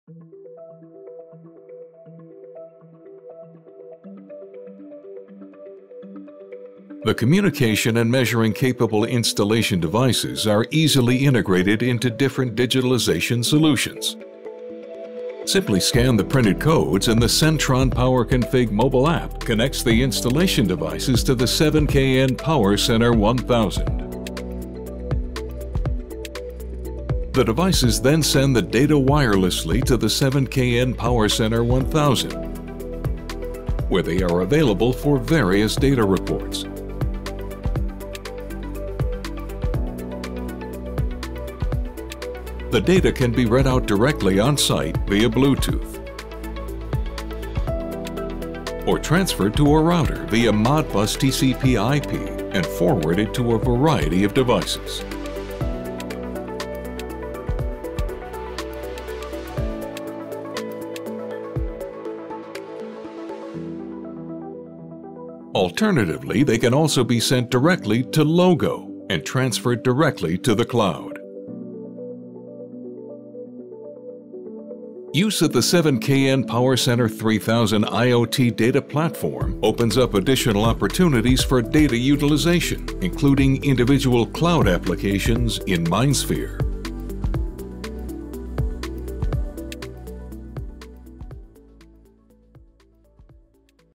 Siemens Sentron Circuit protection devices with Communication functions
Look at what Siemens Sentron Communication devices can do for you,
- Collecting and saving the data and measured values from up to 24 communication-capable circuit protection devices via radio link.
- Parameterization, visualisation and further processing of data in higher-level applications via Bluetooth and Modbus TCP.